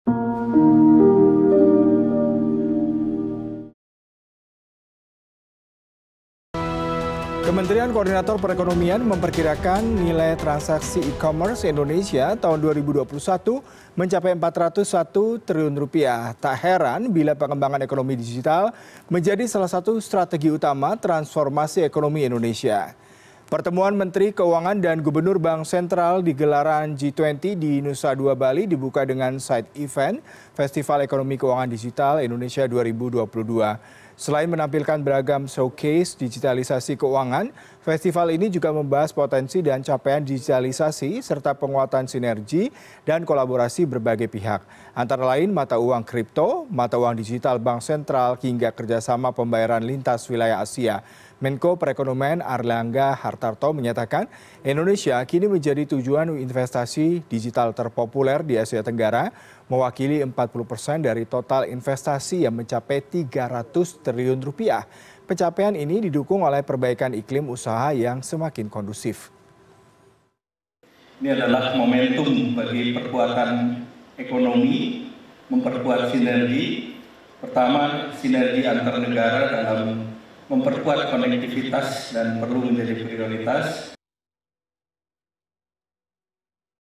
Pertemuan Menteri Keuangan & Gubernur Bank Sentral G20
Kementerian Koordinator Perekonomian memperkirakan, nilai transaksi E Commerce Indonesia tahun 2021 mencapai 401 triliun rupiah. Tak heran bila pengembangan ekonomi digital menjadi salah satu strategi utama, transformasi ekonomi Indonesia.